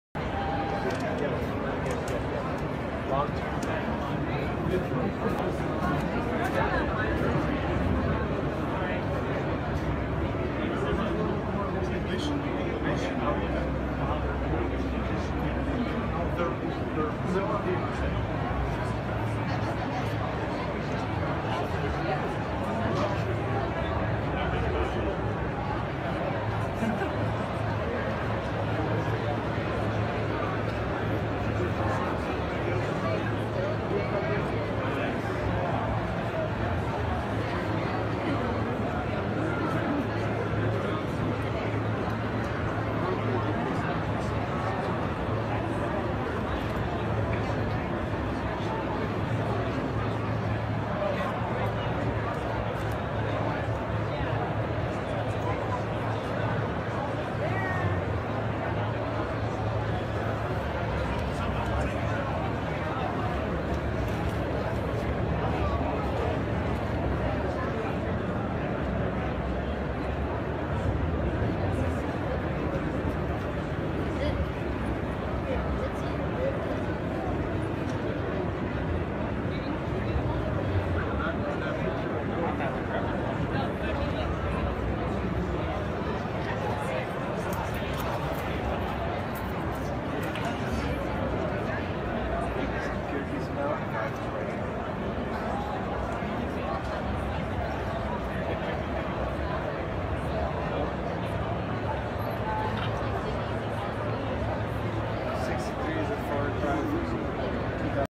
Checking out some history at the HERITAGE AUCTIONS booth.
We walked around the floor doing live interviews.